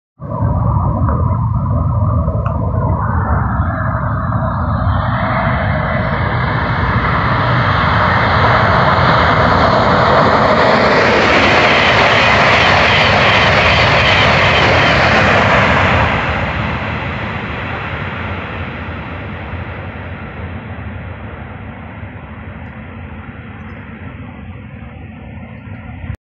ICE 1 Richtung Wendlingen
Auf der SFS Wendlingen-Ulm habe ich mal wieder die Ikone unter den ICE erwischt. Anscheinend gehört er inzwischen zu den Zügen die regelmäßig über die SFS fahren.